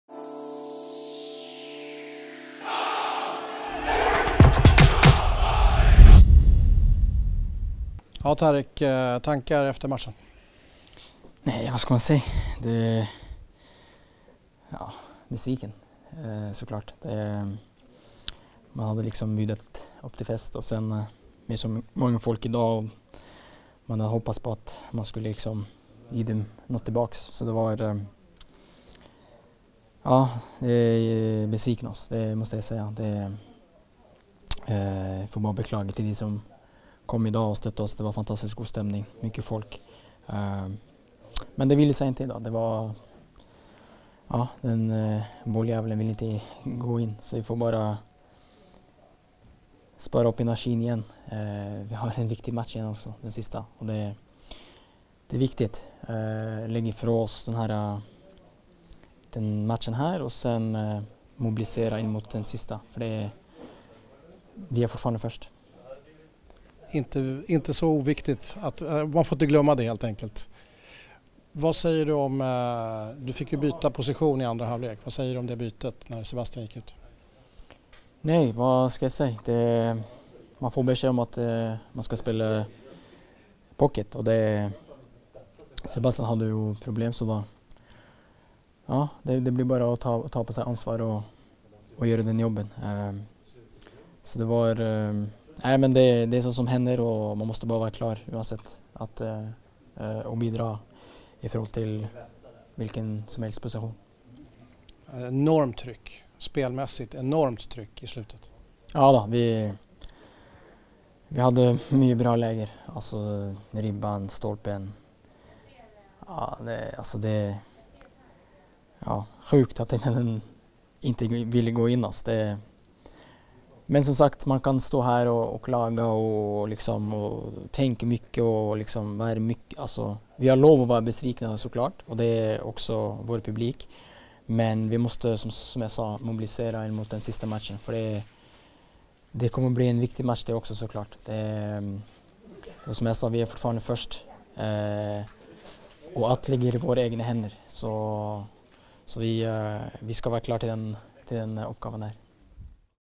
AIK: | 2018-11-04 "Det ligger i våra egna händer" Tarik Elyounoussi efter GIF Sundsvall hemma
Vill du se mer av AIK Fotboll? Följ oss på Sportlib